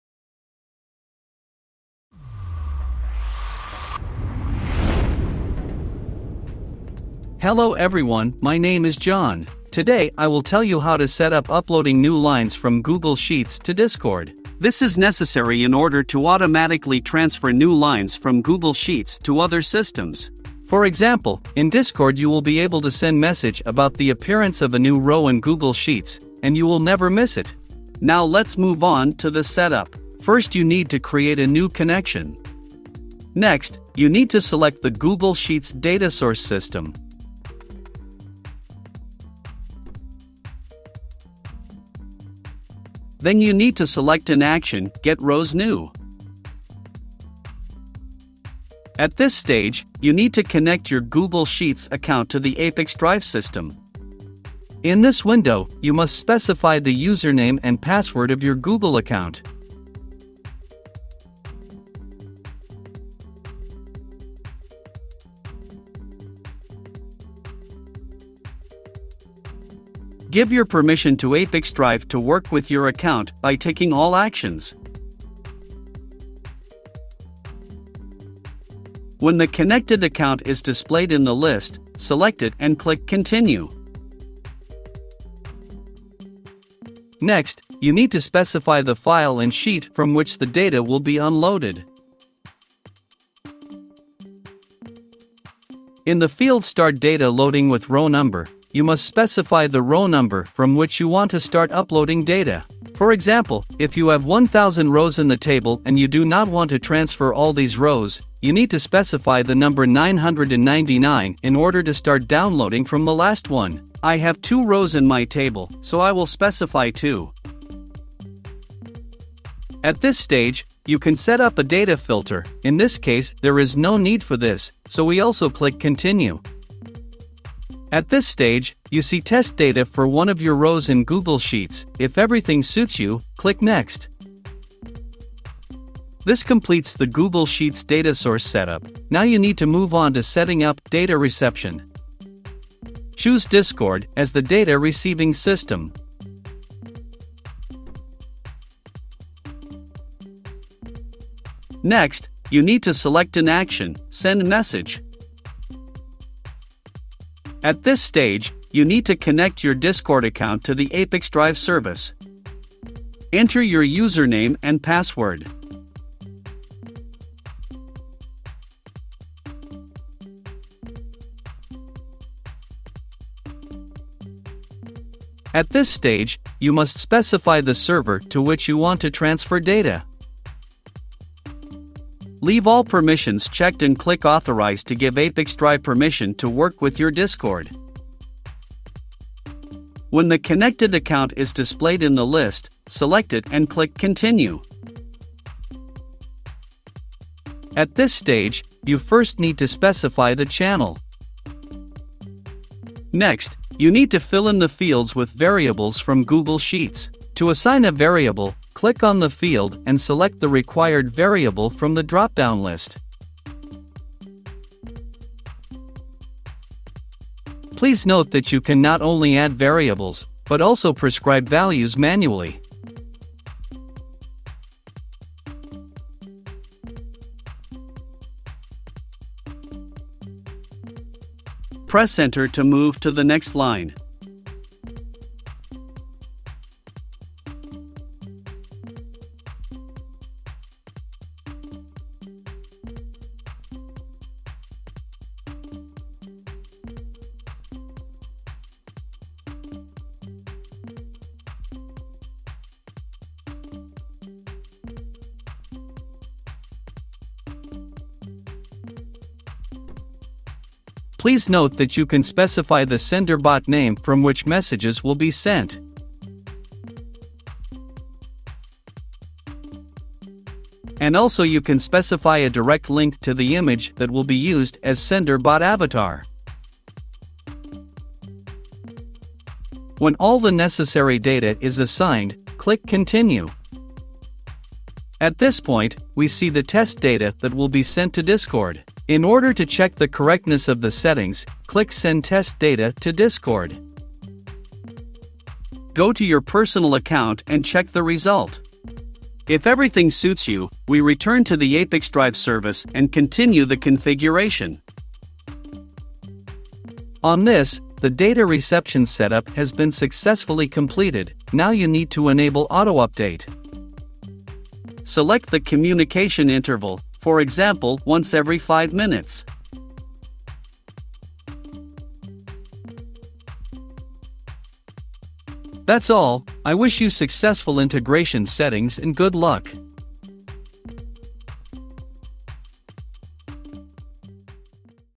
Google Sheets and Discord Integration | How to Get new row from Google Sheets to Discord
How to Get new row from Google Sheets to Discord? Integrate Google Sheets and Discord whith ApiX-Drive

In this video tutorial, we will learn:
✓ How to connect Google Sheets and Discord
✓ How to Send Message from Google Sheets to Discord

【Video Navigation】
0:00 - Introduction
0:30 - Setting up Google Sheets as a Data Source
2:16 - Setting up Discord as a Data Receiver
5:07 - Setting up auto-update

➤  Steps to connect Google Sheets and Discord
1. Link your Google Sheets and Discord  account with ApiX-Drive
2. Select Google Sheets as your data source App and 'Get new row' as your action
3. Select Discord as your data receiver App and 'Send Message' as your action
4. Map Google Sheets fields to relevant fields in Discord
5. Turn your integration on auto update.